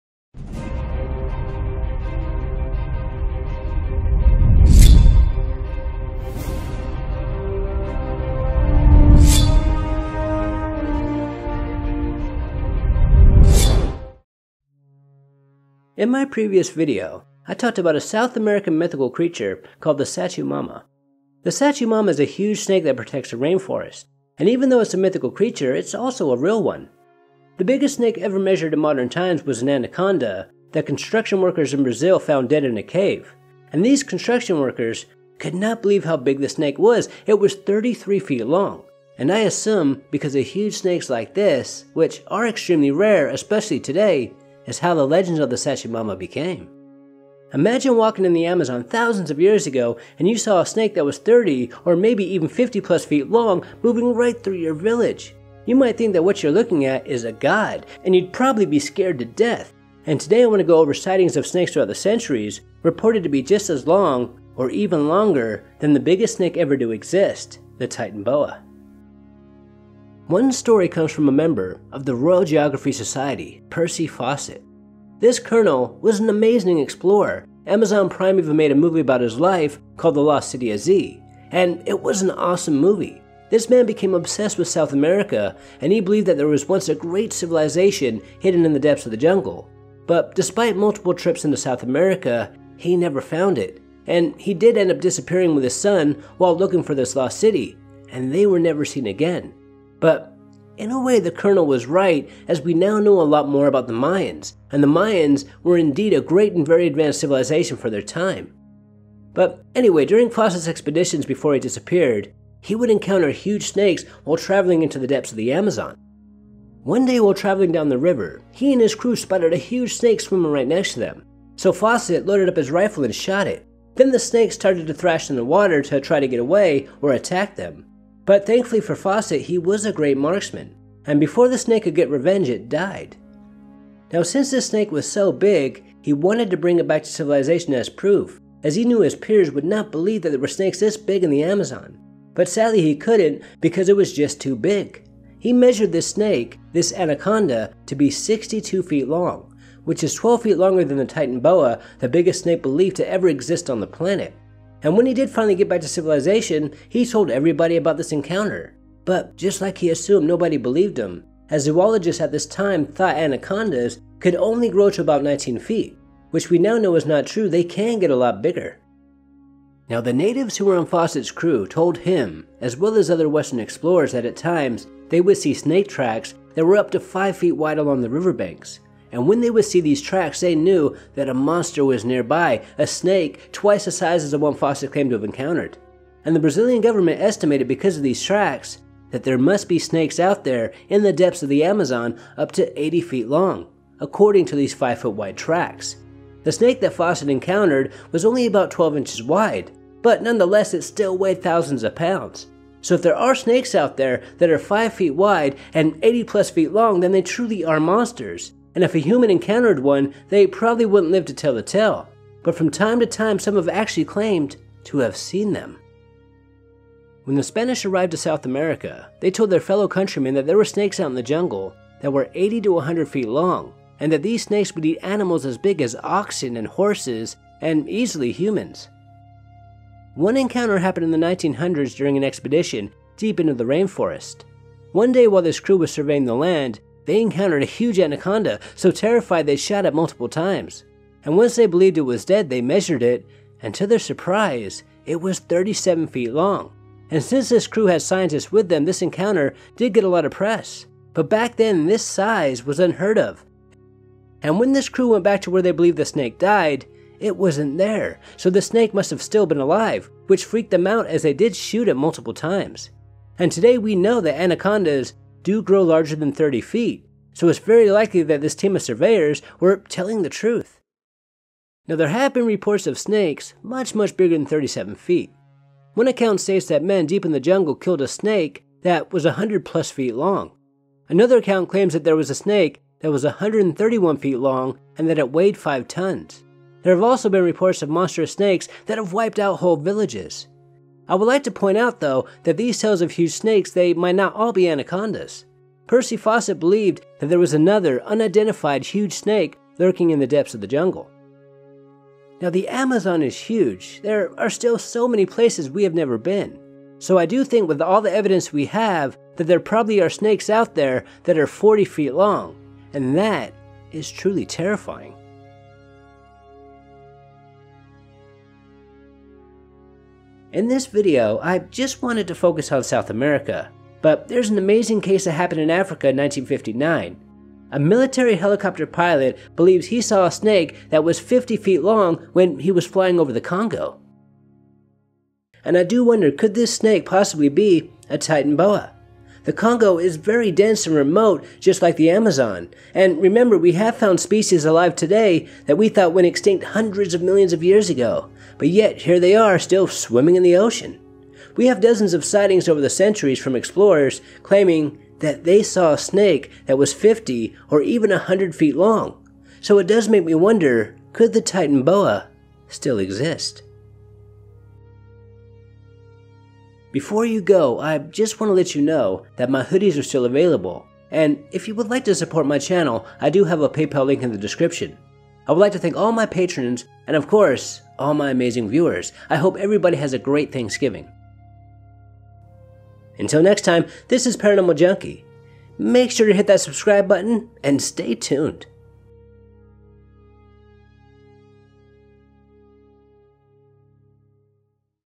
Sightings Of Giant Snakes In The Amazon! Could The Titanoboa Still Exist?
Explorers have long told tails of giant snakes in the depths of the Amazon!